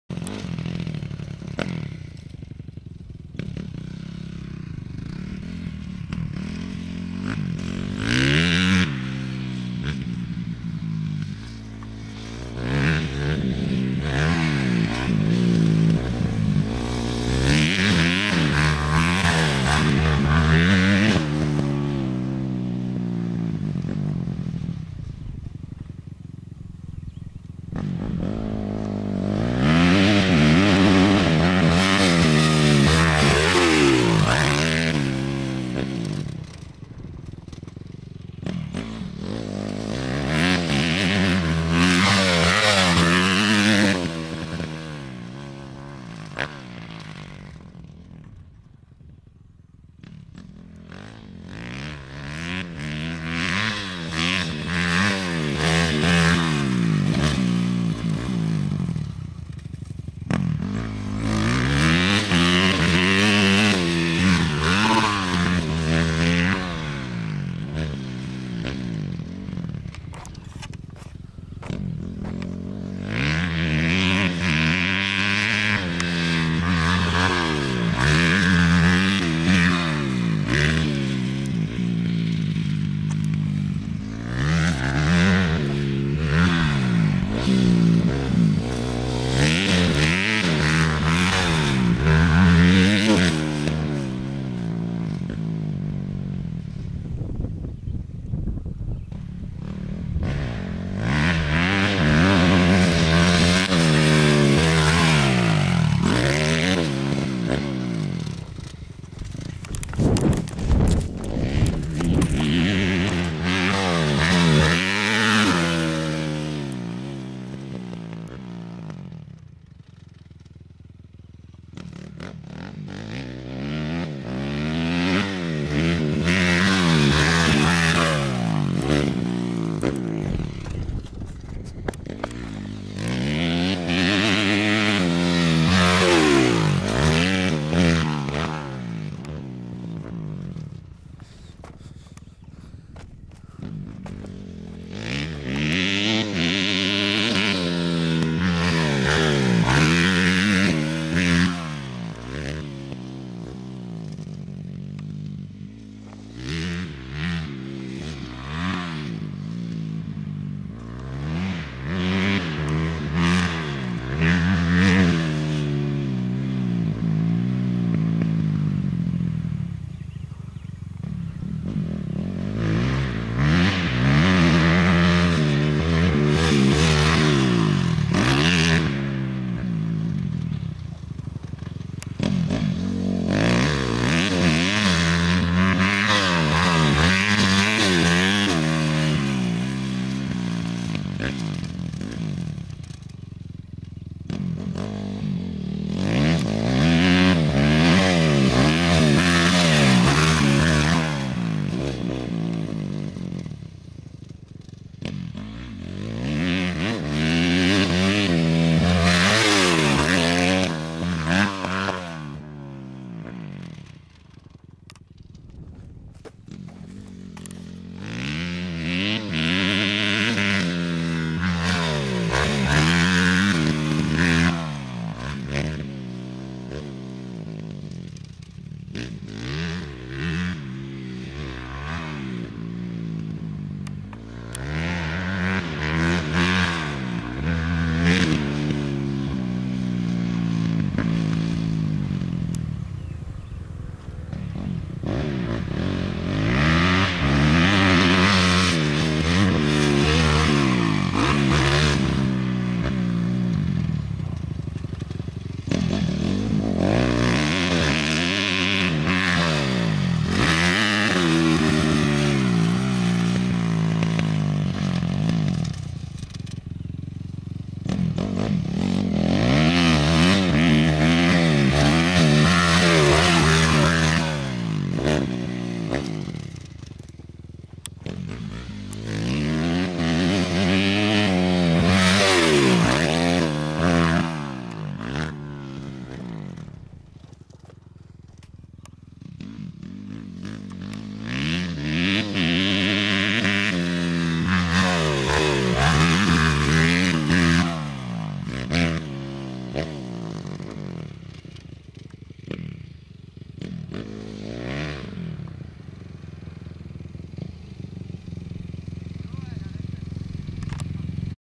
Saint Andre track
this video was recorded at the begining of this summer and it was just the third week of ridding after i had the bike breaked in so i still neded pratice lol